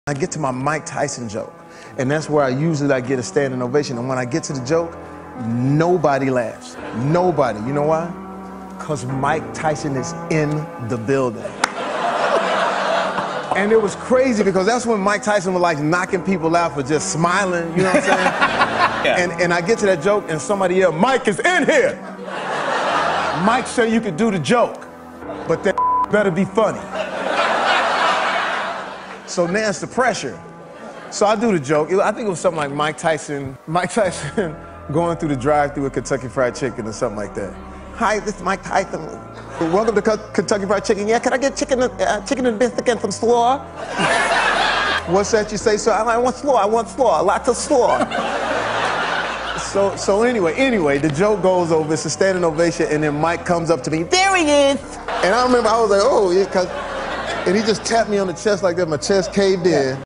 Jamie Foxx: Unveiling the Iron will of Mike Tyson| Youtube Shorts
Delve into the captivating world of boxing legend Mike Tyson as Jamie Foxx takes us on an unforgettable journey, unveiling the raw power and unyielding determination that defined Iron Mike's career. Join us for an exclusive peek behind the scenes as Foxx immerses himself in the role, bringing Tyson's larger-than-life persona to the screen with unparalleled authenticity and depth. Get ready to witness the untold story of one of the greatest athletes of all time in this YouTube Shorts special. Don't miss a moment of the action!"